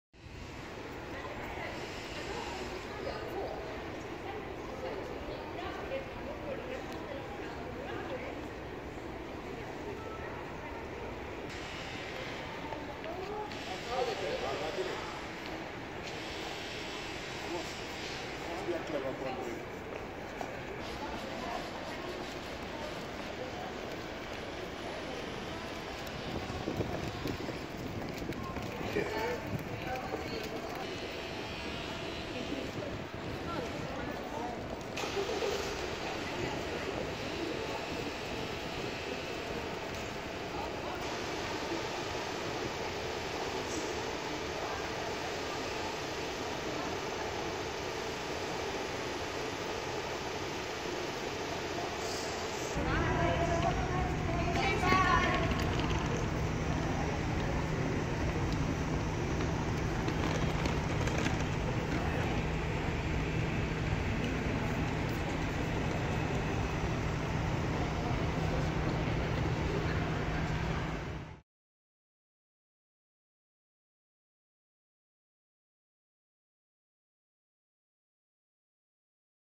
Police tape surrounds Louis Vuitton in Manchester after break in
A Louis Vuitton store in Manchester's city centre where handbags sell for more than £2,000 was raided in the early hours of this morning (Pictured store front left this morning and, right, bags strewn outside the shop in Manchester). Its side window was smashed in and pictures from the scene show boxes strewn across the ground outside. A sledge-hammer has also been found lying on the ground outside the luxury clothing store(inset). It is unclear at this stage what was taken from the store.